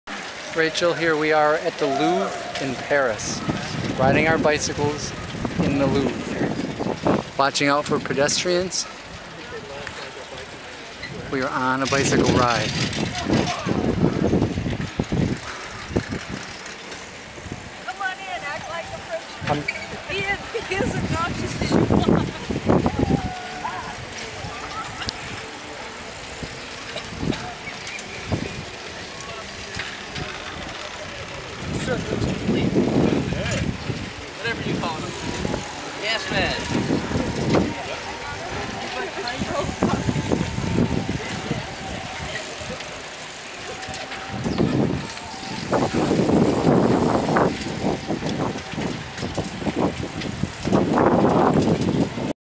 Fat Tire bicycle tour - Paris 3
Fat Tire bicycle tour - Paris.  Great fun!! Excellent hosts.
Louvre fountain was magical...